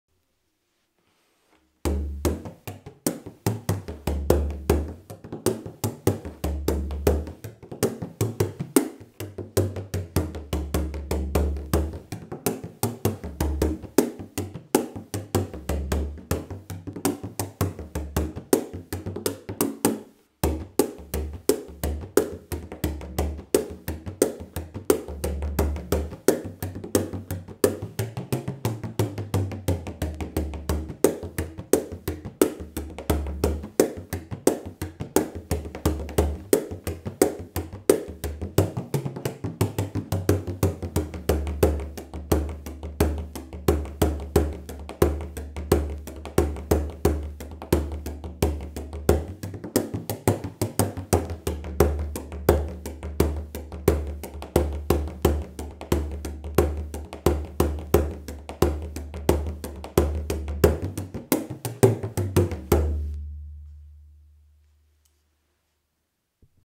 Sound sample of 13" bodhran by Job Cornelissen
Birch plywood, Walnut Veneer, Goat skin cured by Brendan White, 5 Metloef/ Bartlett tuners. Please use earphones for listening!